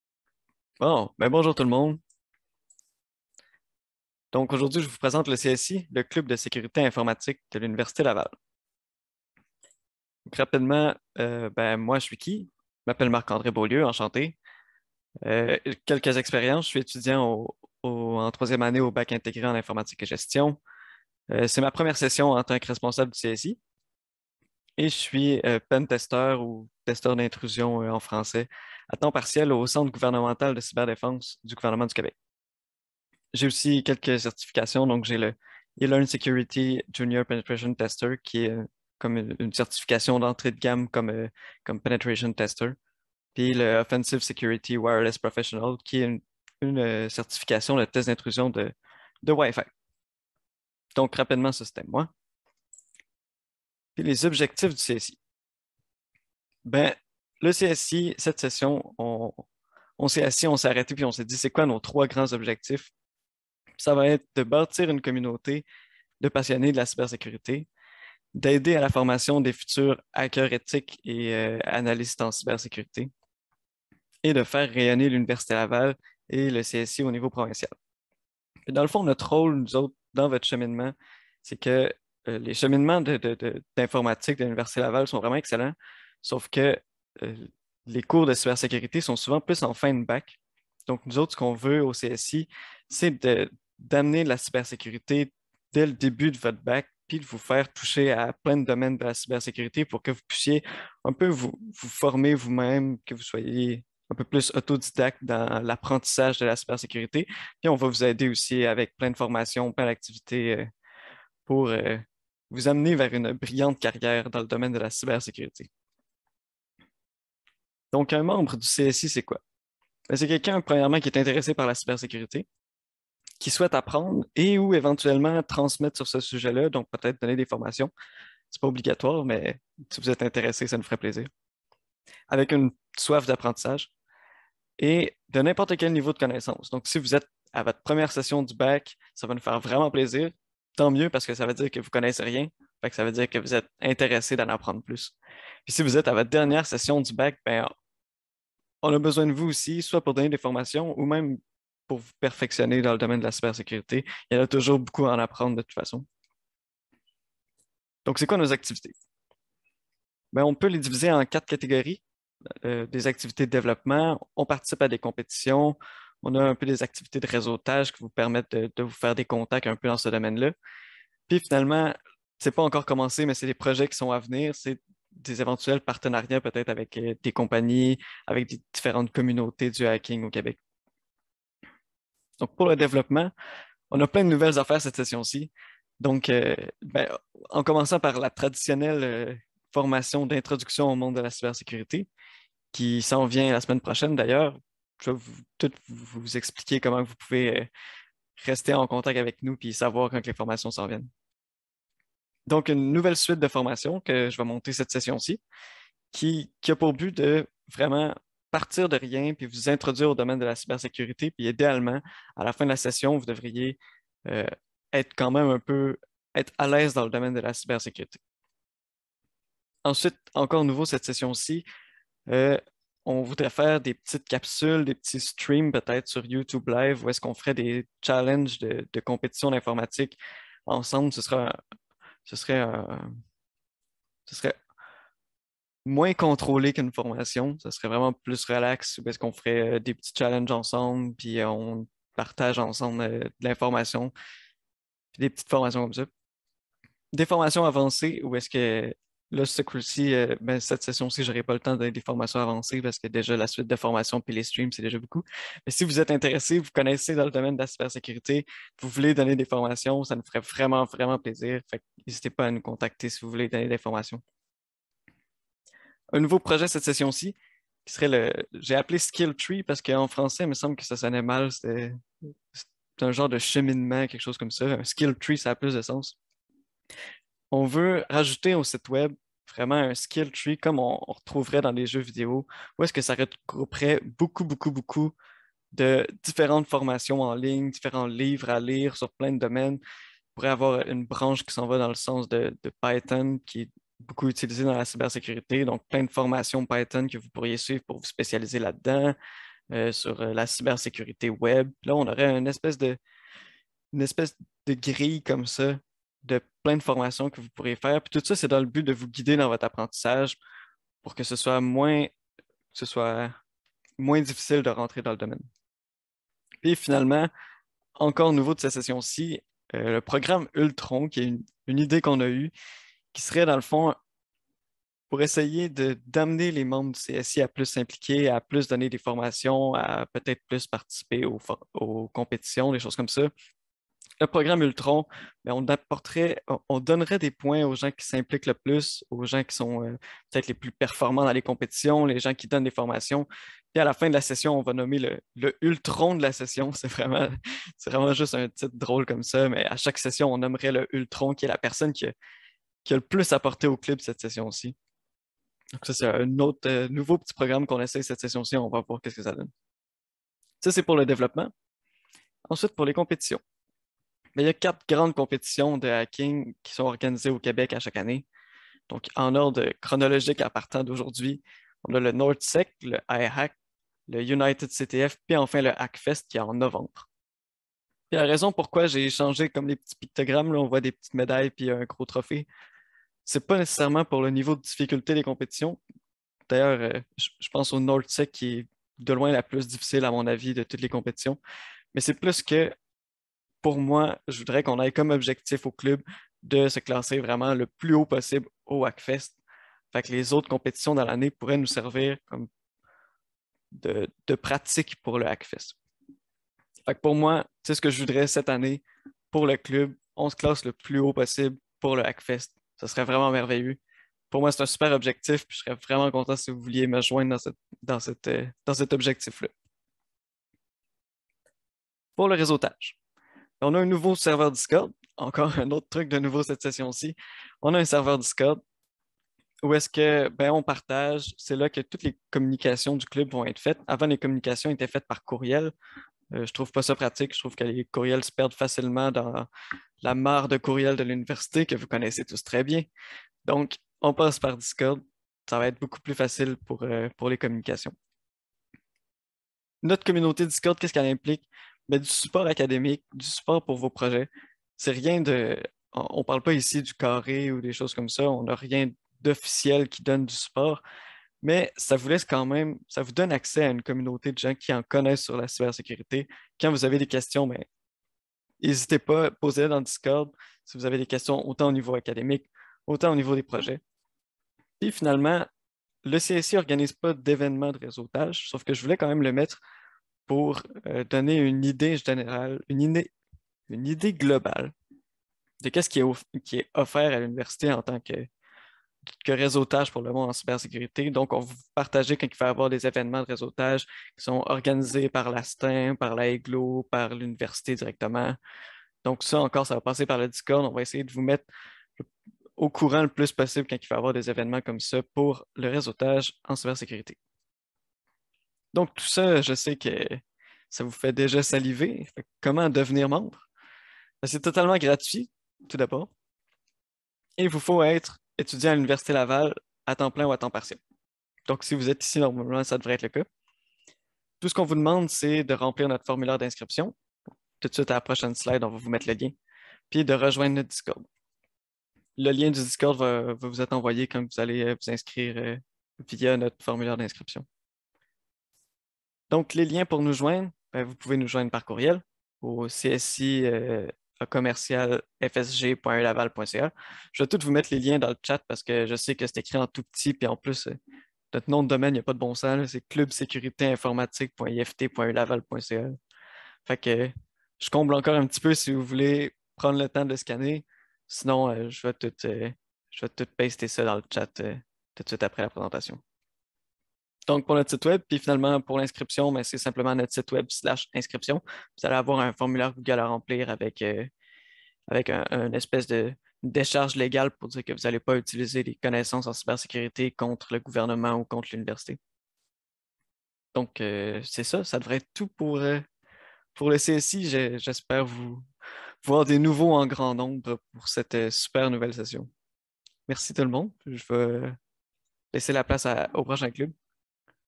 Présentation du CSIUL - H2022
Présentation du Club de sécurité informatique de l'Université Laval pour la session d'hiver 2022.